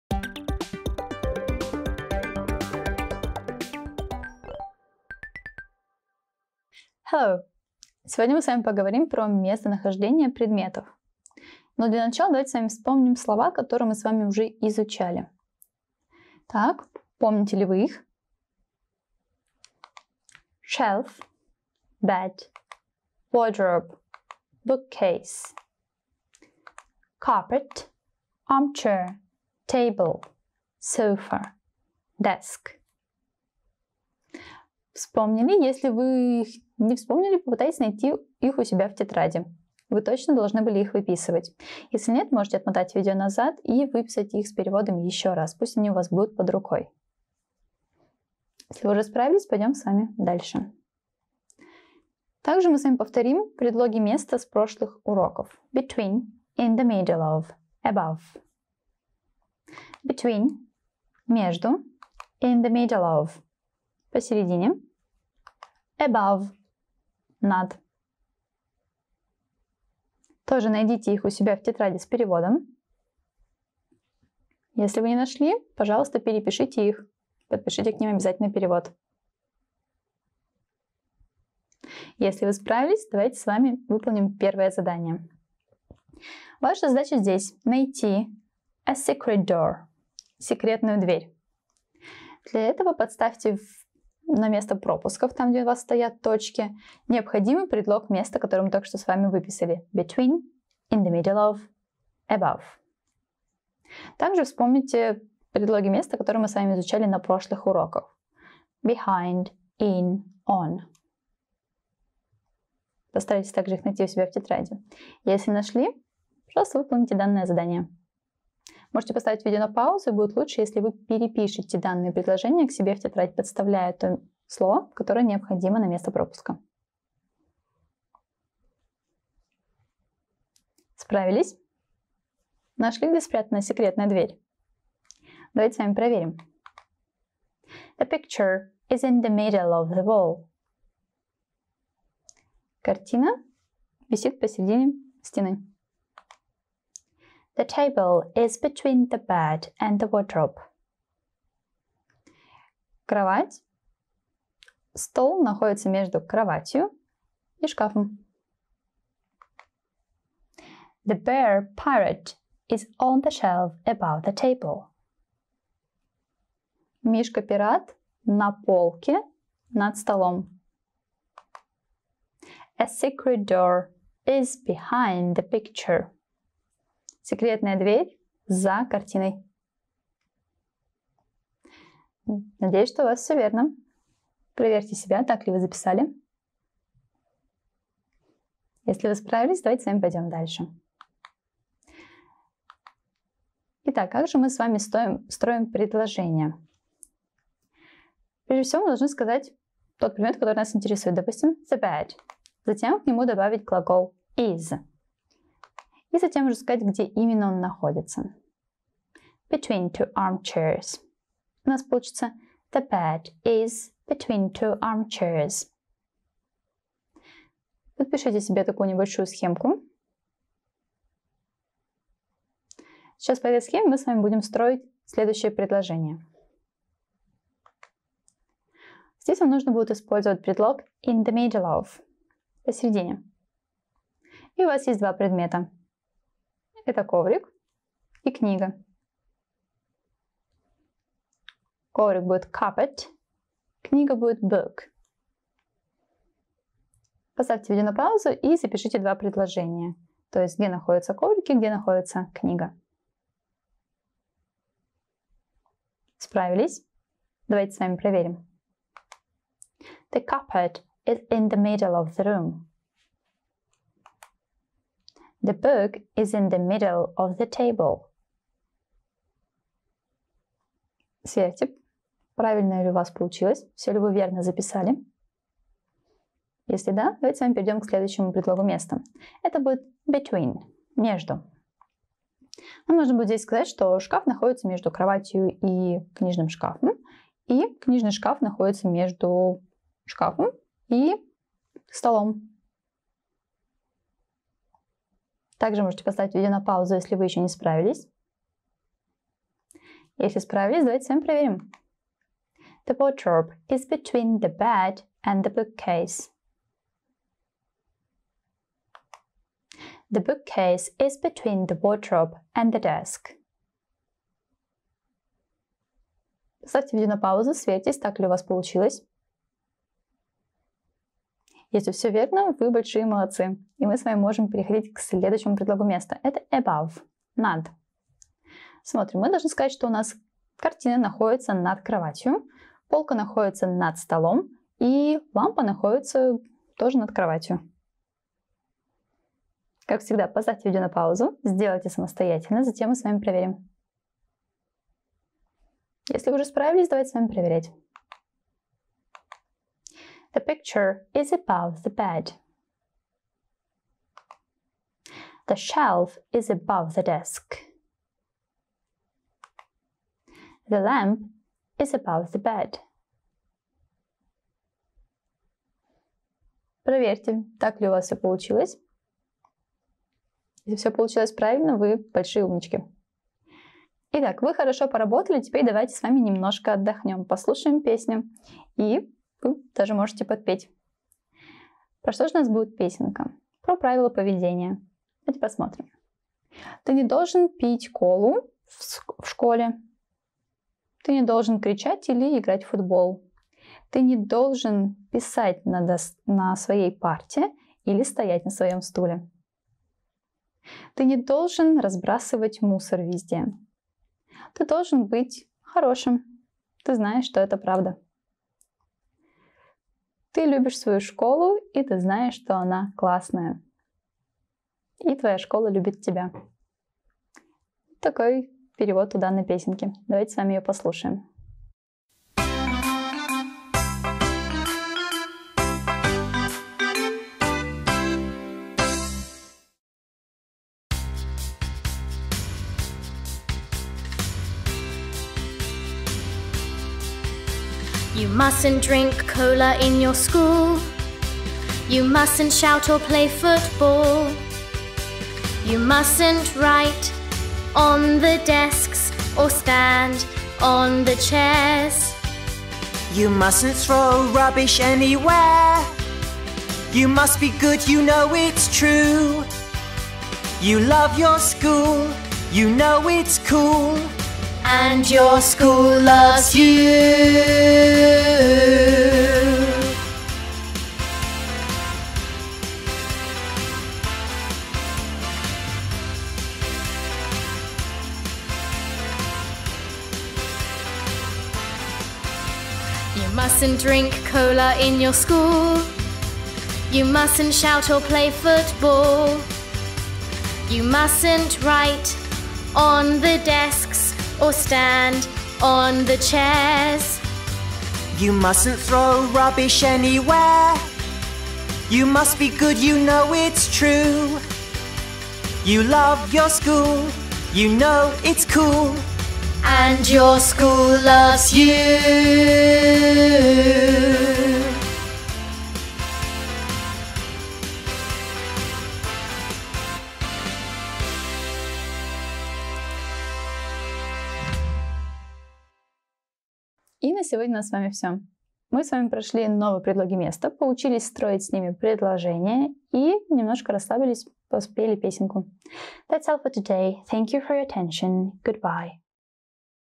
Видеоурок 35. Описание местонахождения предметов. Английский язык 2 класс
План урока:
• повторим слова;
• повторим предлоги места: between, in the middle of, above;
• выполним задание
• послушаем песенку.